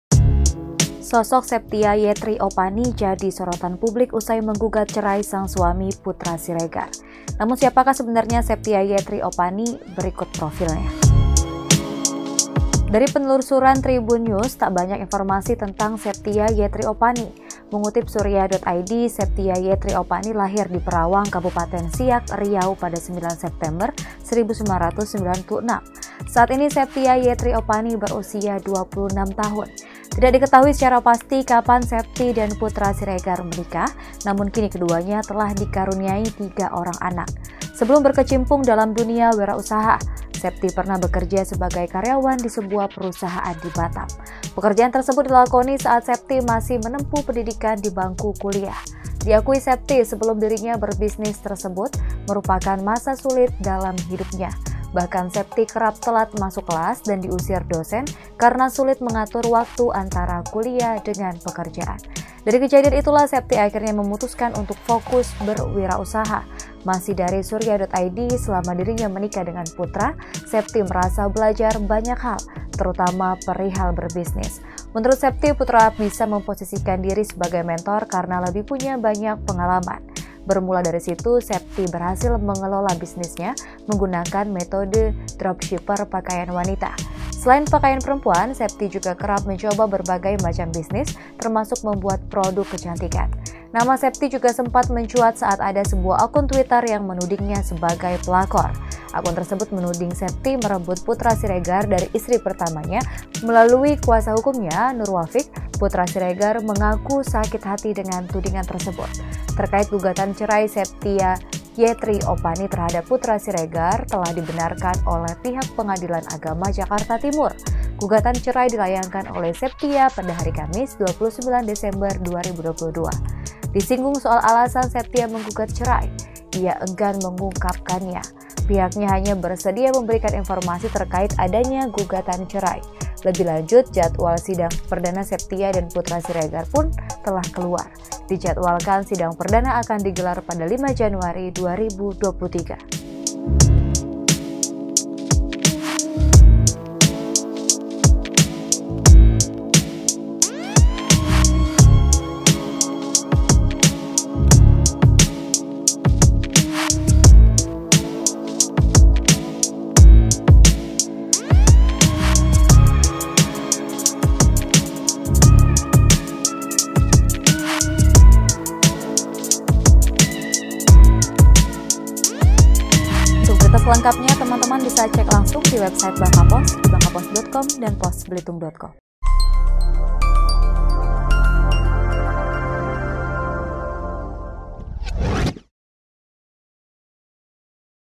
Profil Septia Yetri Opani yang Gugat Cerai Putra Siregar, Pernah Dituding Pelakor dan Diusir Dosen
Sosok Septia Yetri Opani jadi sorotan publik usai menggugat cerai sang suami, Putra Siregar.
Pasalnya, selama ini Putra Siregar dan Septia Yetri Opani tampak harmonis.
Lantas, siapa sebenarnya sosok Septia Yetri Opani ini?